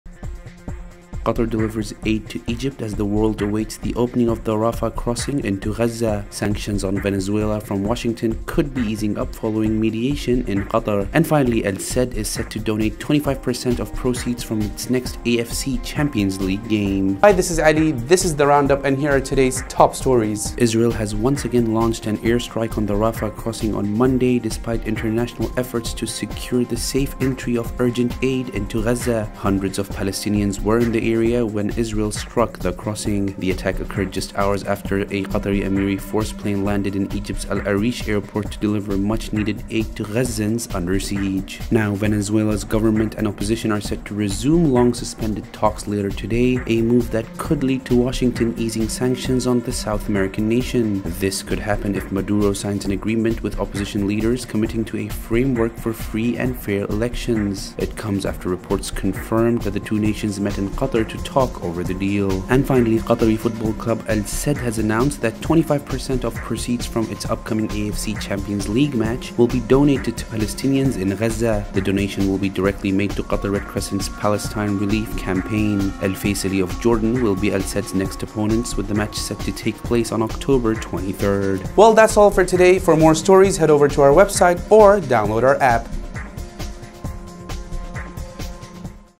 Venezuela to speak with opposition & other top stories
Top stories on TheRoundup today

Qatar delivers aid to Egypt for Gaza
Venezuela-US mediation in Qatar
Al Sadd steps up for Palestine